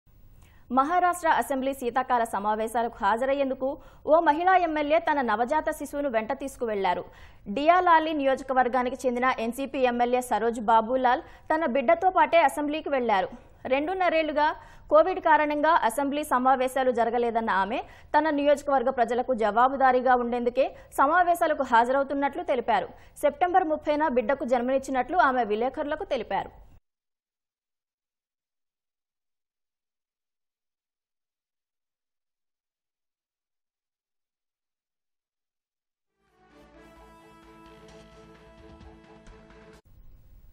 NCP Lawmaker Saroj Ahir Attends Maharashtra Assembly Session With Baby |బిడ్డతోఅసెంబ్లీకి హాజరైన MLA
మహారాష్ట్ర అసెంబ్లీ శీతాకాల సమావేశాలకు. హాజరయ్యేందుకు ఓ మహిళా ఎమ్మెల్యే తన నవజాత శిశువును వెంట తీసుకువెళ్లారు. డియాలాలి నియోజకవర్గానికి చెందిన ఎన్సీపీ ఎమ్మెల్యే సరోజ్ బాబూలాల్ ... తన బిడ్డతో పాటే అసెంబ్లీకి వెళ్లారు. రెండేన్నరేళ్లుగా కోవిడ్ కారణంగా అసెంబ్లీ సమావేశాలు జరగలేదన్న ఆమె.. తన నియోజకవర్గ ప్రజలకు జవాబుదారీగా ఉండేందుకే సమావేశాలకు హాజరవుతున్నట్లు తెలిపారు. సెప్టెంబర్ 30న బిడ్డకు జన్మనిచ్చినట్లు ఆమె విలేఖరులకు తెలిపారు.